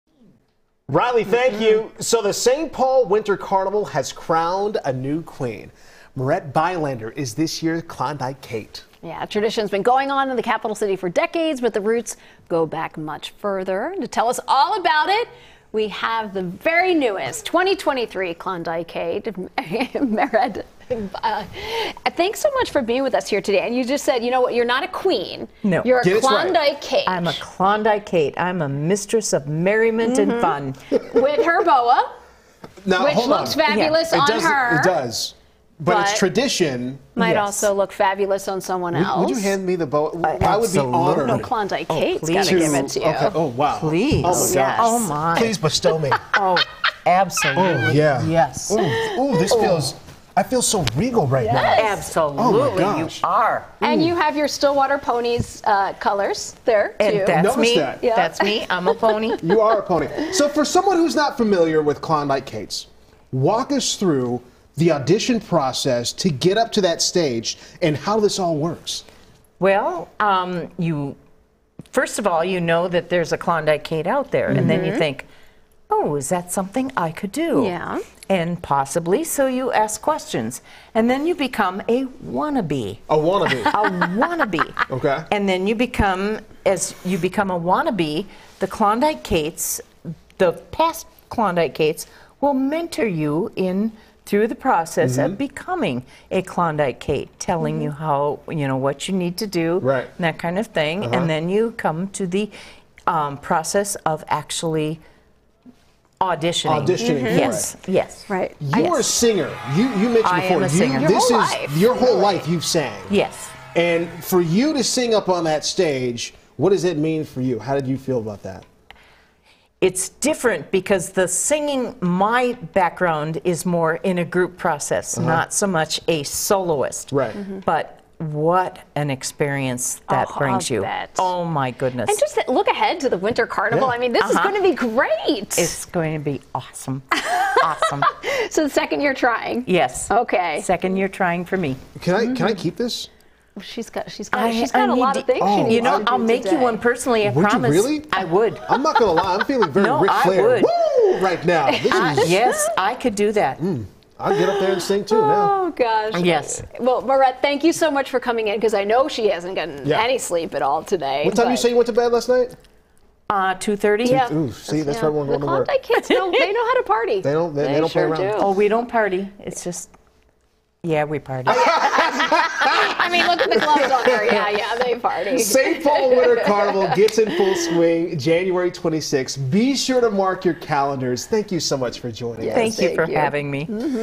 Maret Bylander selected as 2023's Klondike Kate
The St. Paul Winter Carnival has crowned a new queen!